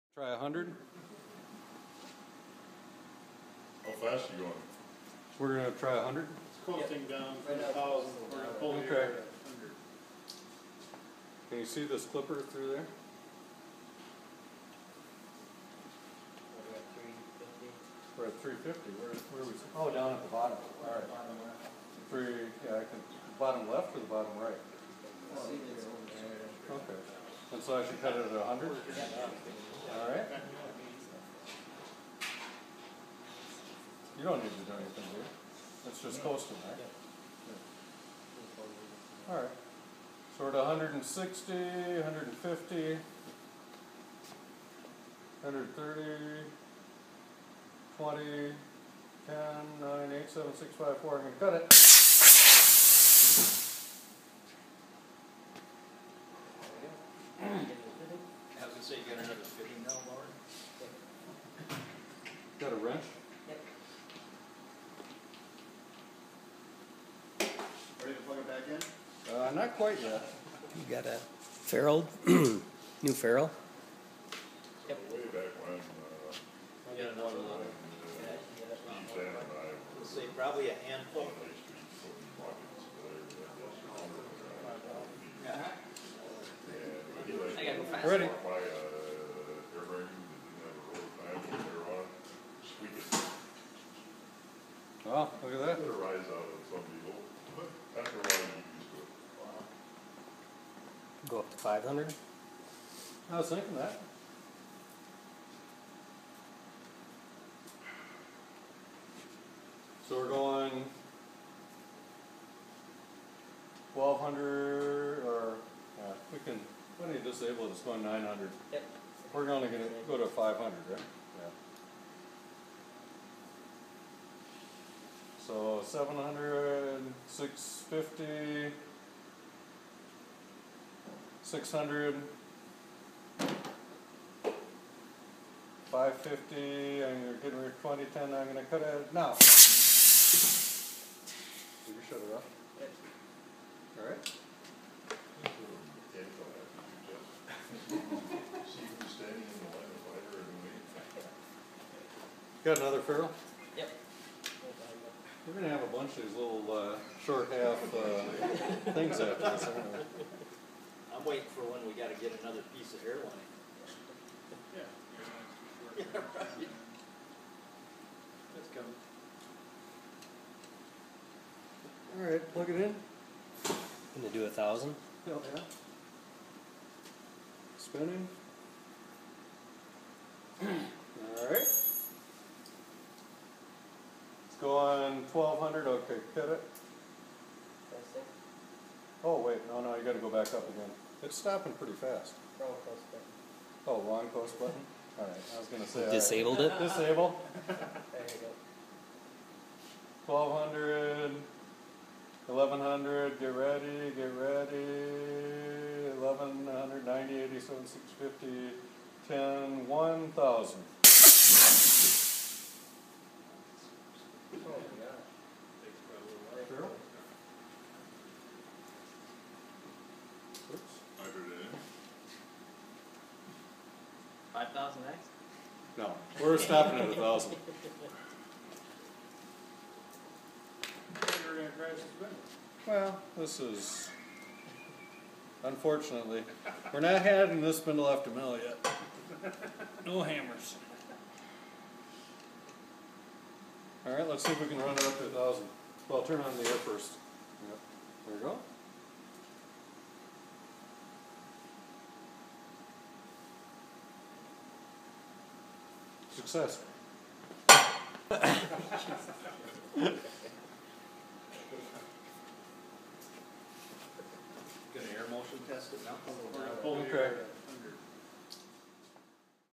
Crash Testing an Experimental Porous ISO 5.5 Air Bearing
Experimental ISO 5.5 with porous compensation. We cut the air at 100, 500 and 1,000 rpm, and the spindle still spins free. The chuck on the front of the spindle is from Innolite.